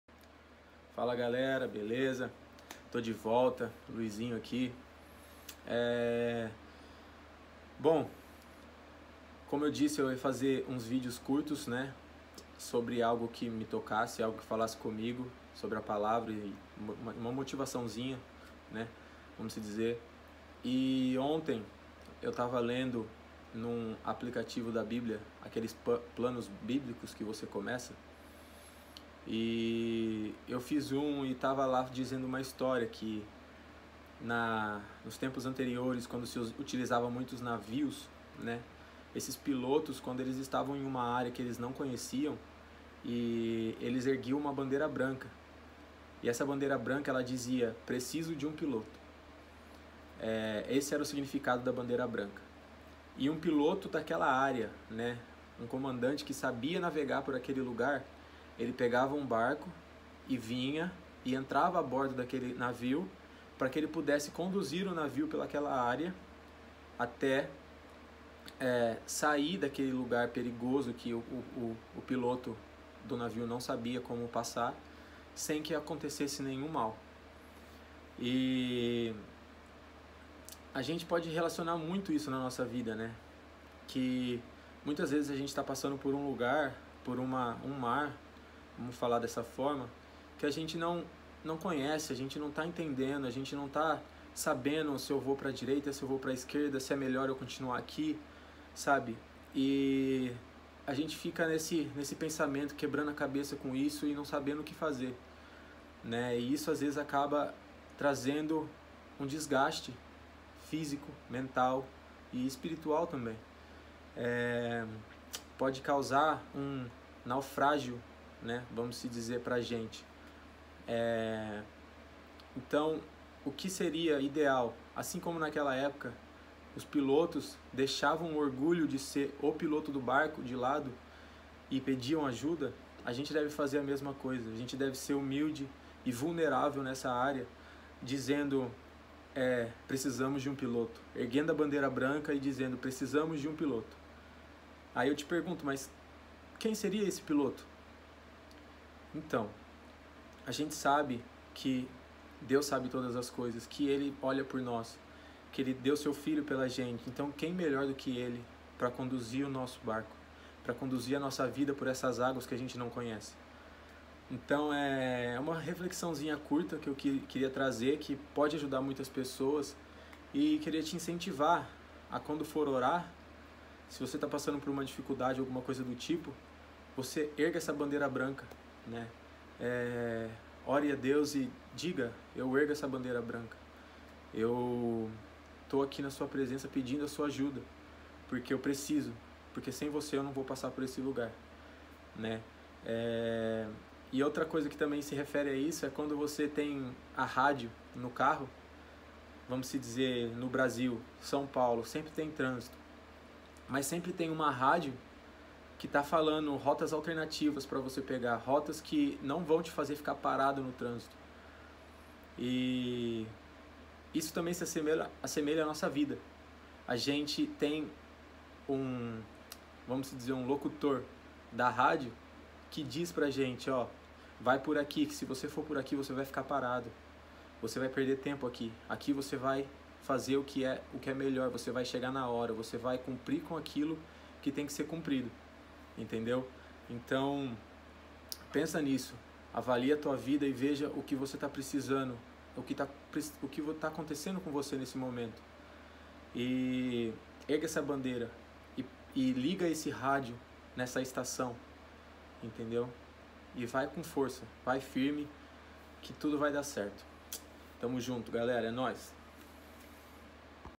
ERGA A BANDEIRA BRANCA!
Fala galera, como voces estão? Hoje to começando essa série de videos de "motivação" usando a palavra de Deus.

Da uma curtida , comenta e compartilha pra ajudar o canal e a levar a palavra de Deus pra mais pessoas. Tamo junto galerinha.